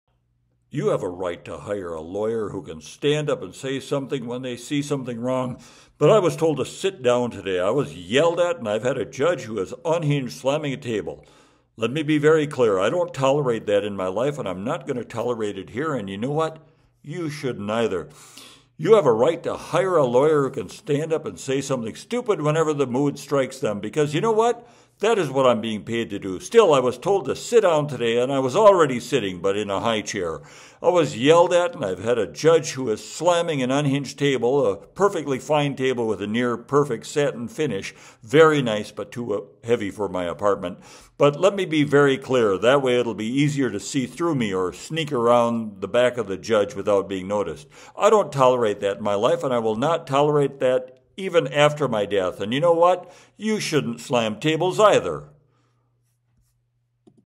Mangy Fetlocks Channels Alina Habba
Here, Mangy Fetlocks, known for his lyric genius, but hardly known at all for his great psychic talents, channels the great Alina Habba, Trump's remarkably below-average lawyer.

You have a right to hire a lawyer who can stand up and say something when they see something wrong, but I was told to sit down today. I was yelled at and I’ve had a judge who is unhinged slamming a table. Let me be very clear.  I don’t tolerate that in my life. I’m not gonna tolerate it here, and you know what?  you shouldn’t either.

You have a right to hire a lawyer who can stand up and say something stupid whenever the mood strikes them, because, you know what? that is what I’m being paid to do.  Still, I was told to sit down today, and I was already sitting, but in a high chair.  I was yelled at and I’ve had a judge who is slamming an unhinged table, a perfectly fine table with a near perfect satin finish.  Very nice, but too heavy for my apartment, but let me be very clear. That way it will be easier to see through me, or sneak around that back of the judge without being noticed. I don’t tolerate that in my life, and I will not tolerate that after my death, and you know what?,  you shouldn’t slam tables either.